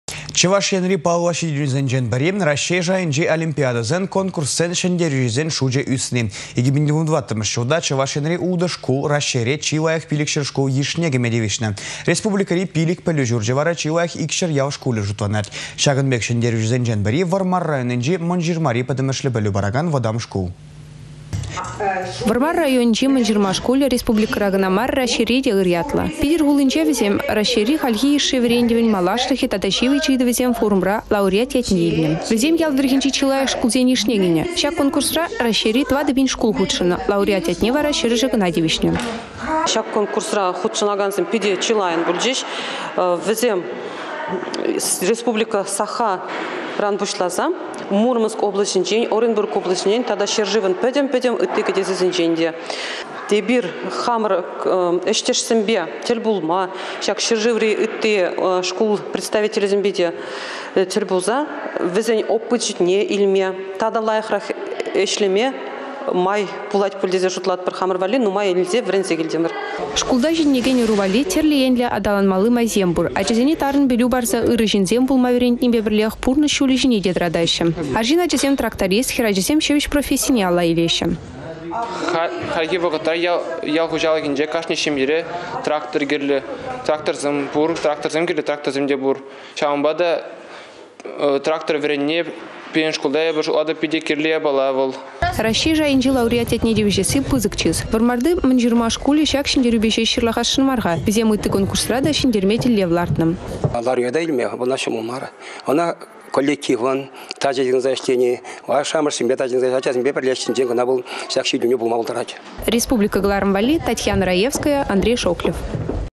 Мăнçырма шкулĕ Раççейри чи лайăх ял шкулĕ шутланать
Чăваш Енри паллă çитĕнÿсенчен пĕри - Раççей шайĕнчи олимпиадăсен, конкурссен çĕнтерÿçисен шучĕ ÿсни. 2014-мĕш çулта Чăваш Енри 6 шкул Раççейре чи лайăх 500 шкул йышне  кĕме тивĕçнĕ. Республикăри 5 пĕлÿ çурчĕ вара чи лайăх 200  ял шкулĕ шутланать. Çакăн пек çĕнтерÿçĕсенчен пĕри Вăрмар районĕнчи Мăнçырмари пĕтĕмĕшле пĕлÿ паракан вăтам шкул.